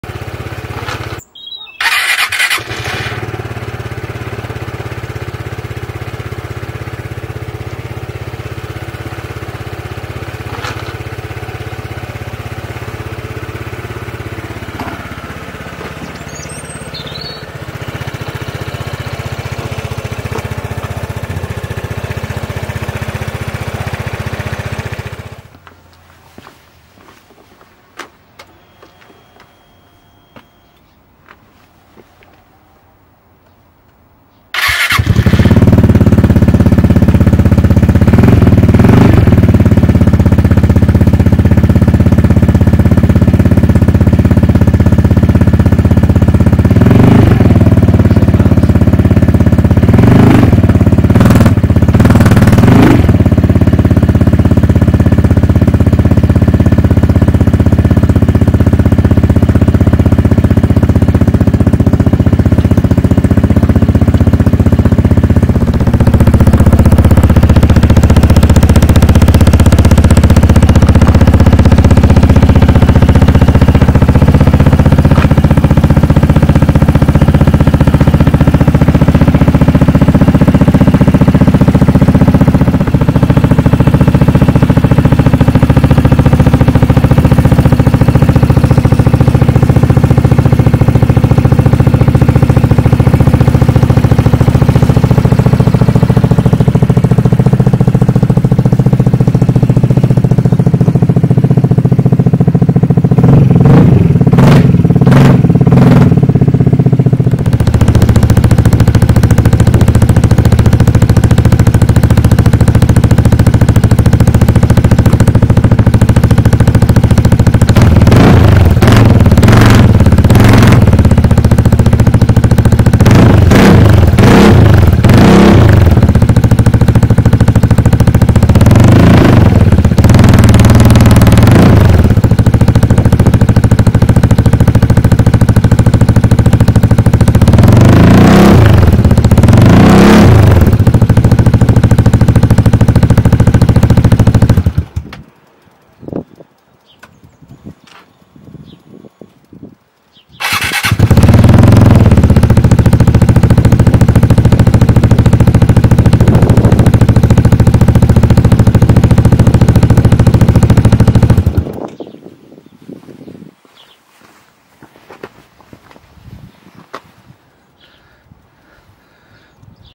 Vespa GTS 300 HPE Scorpion Red Power HD 720p
Comparison of Standard exhaust and Scorpion Red Power Exhaust on my 2020 Vespa 300 GTS HPE Euro 4